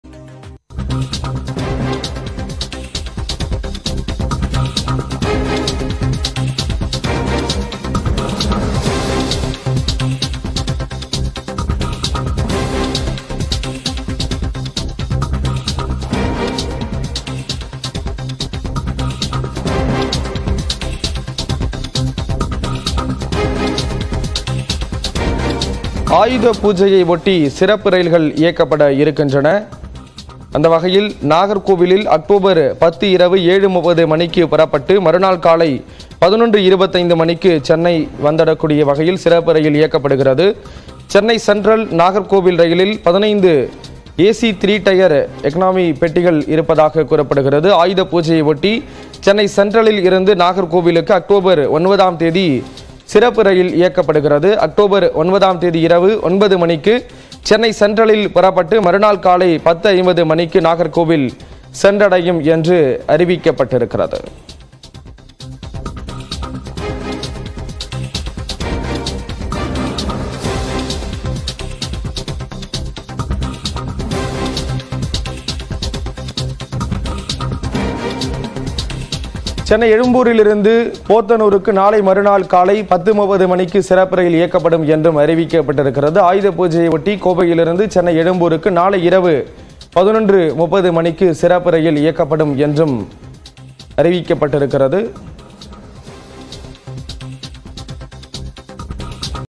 Indian Railway | கோவை - சென்னை எழும்பூருக்கு நாளை சிறப்பு ரயில் | Ayudha Poojai
அக். 9 இரவு 9 மணிக்கு சென்னை சென்ட்ரலில் புறப்பட்டு மறுநாள் காலை 10.50 மணிக்கு நாகர்கோவிலை சென்றடையும்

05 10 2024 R

list=PLZjYaGp8v2I8Jl8PfHo2MQdWKs42-15cX